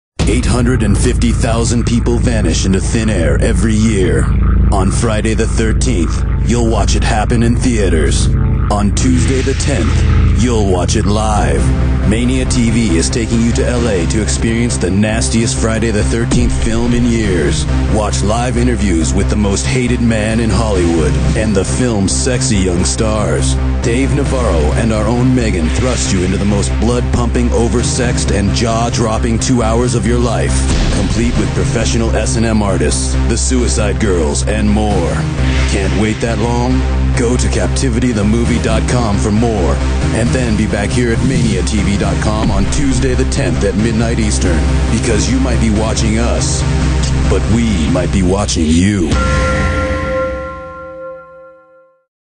Captivity Release Party Promo
Here is the Promotional clip for the Captivity Release Party that will be broadcast live on ManiaTV.com